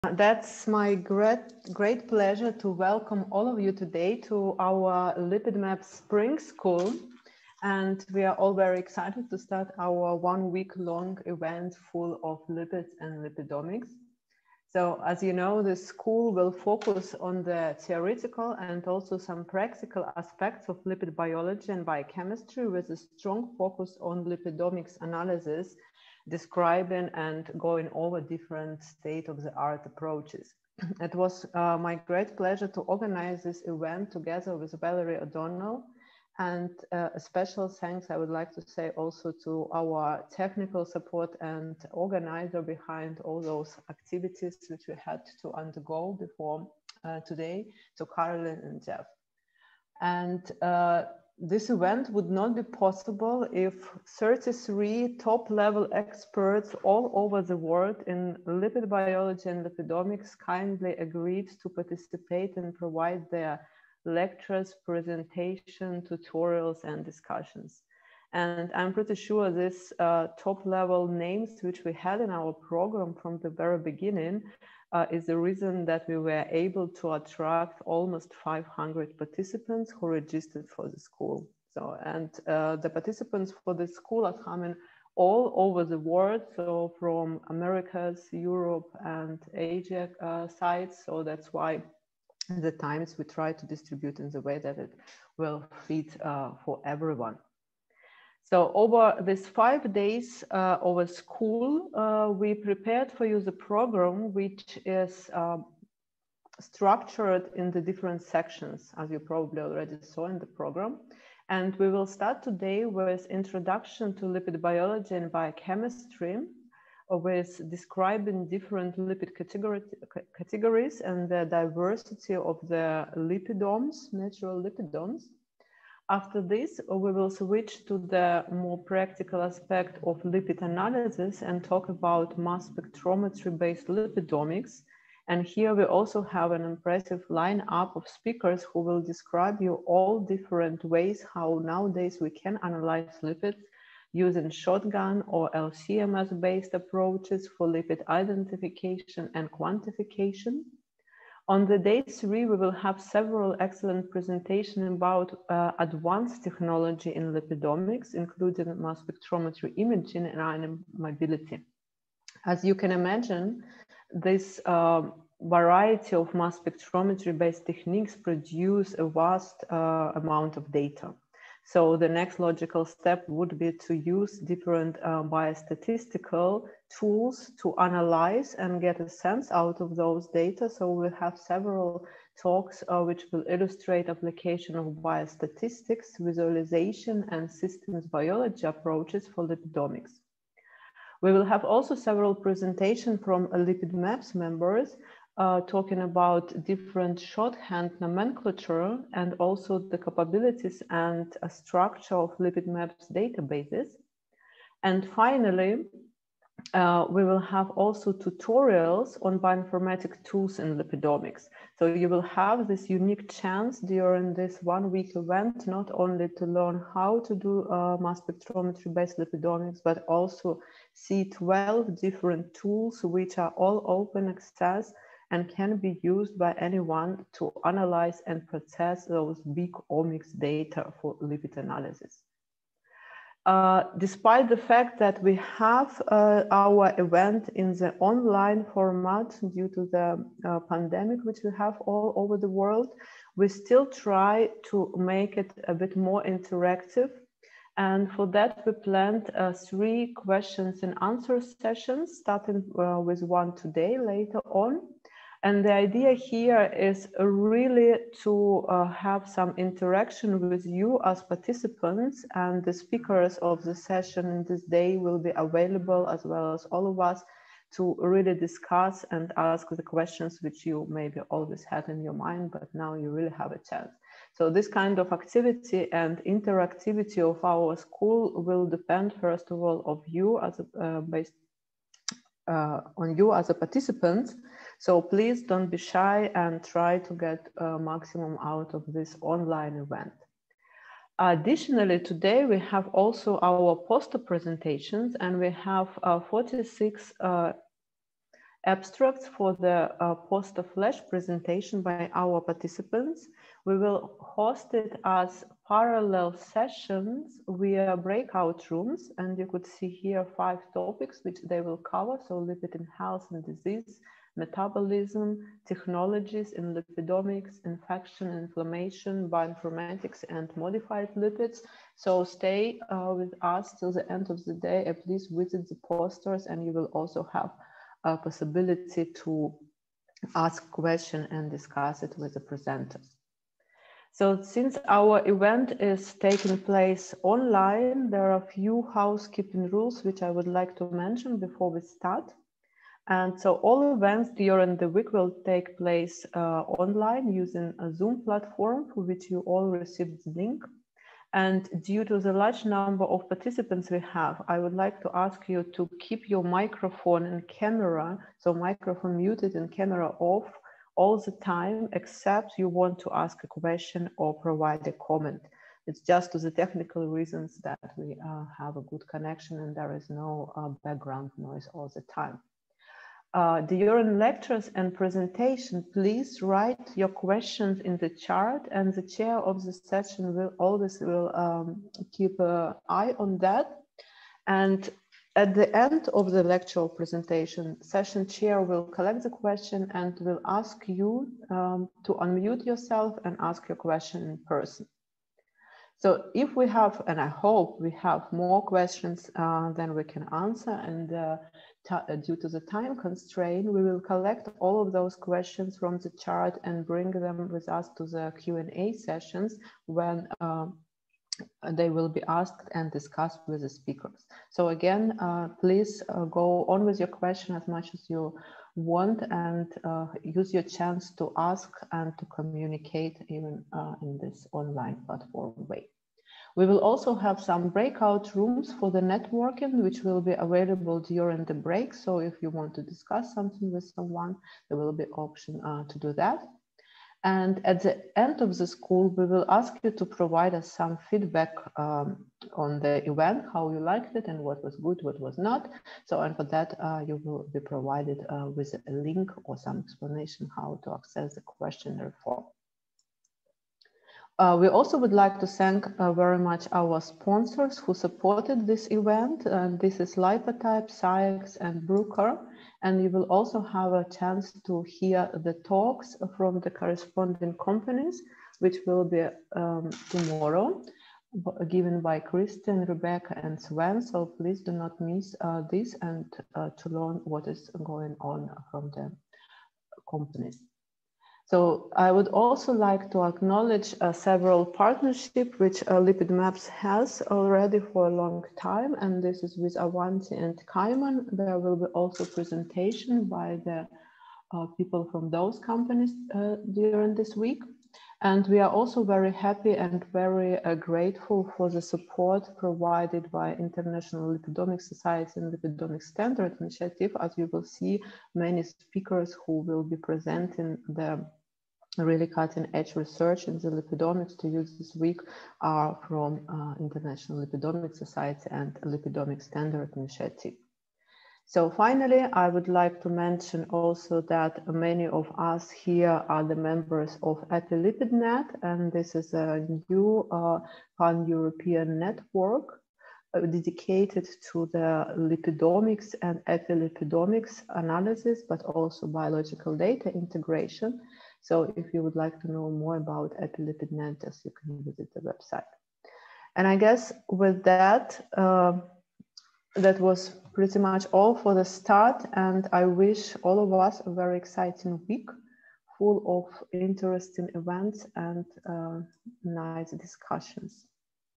Maria Fedorova- Welcome to participants and Introduction to the School
Presented by Maria Fedorova at Lipid Maps Spring School 2021.